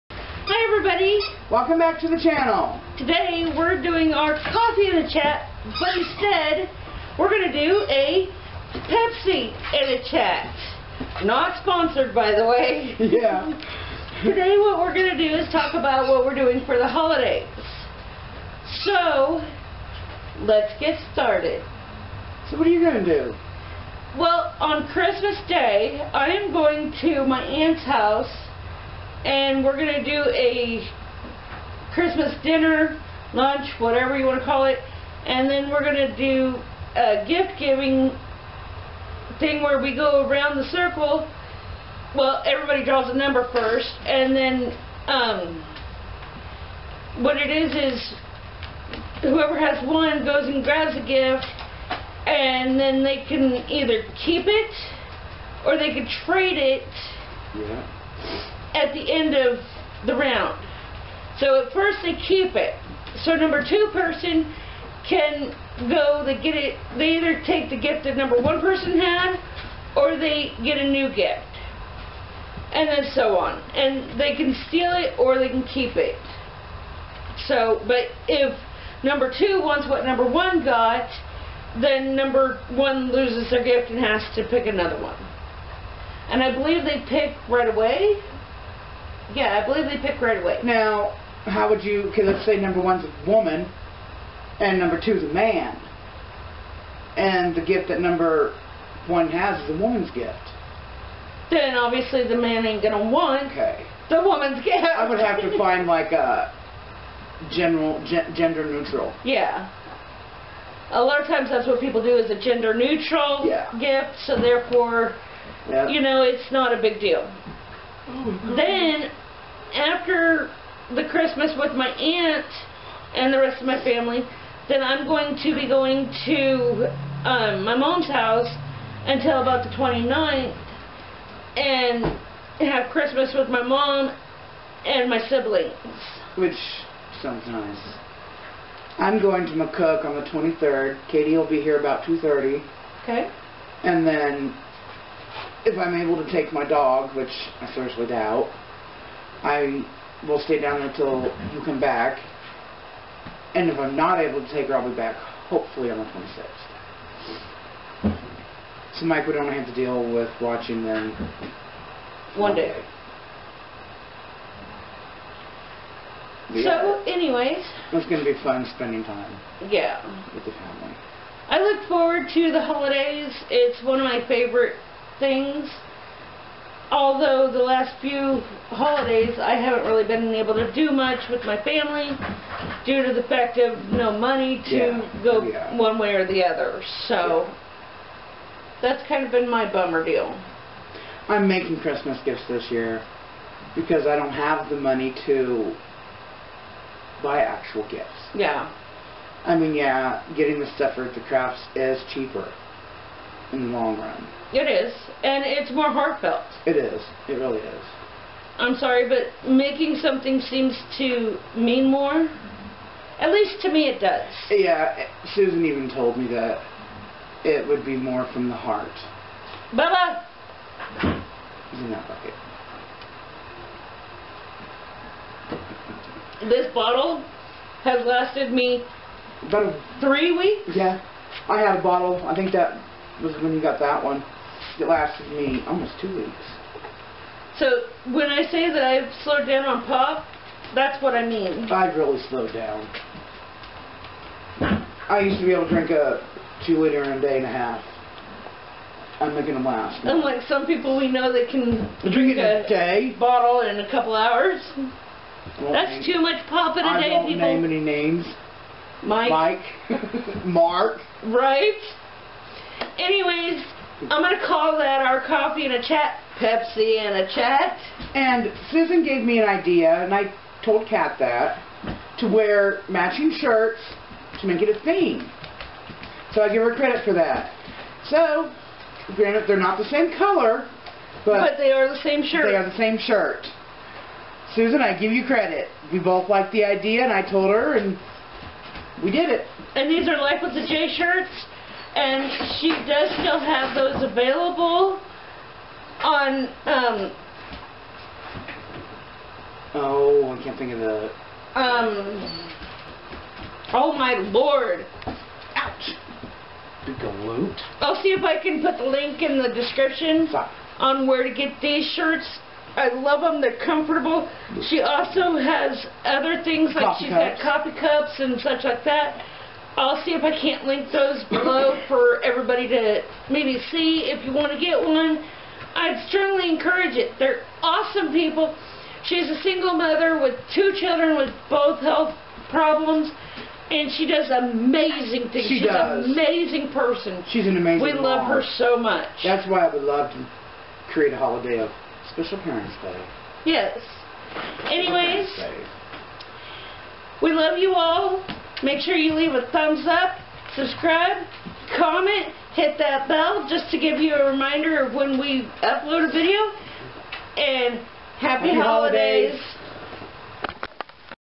Coffee and a Chat # 4 What we are doing for the Holidays
Here we done what we are doing for the holidays. We hope you are all still enjoying these videos, if you are let us know what videos are your favorite?
here is the link for Life with the J's shirts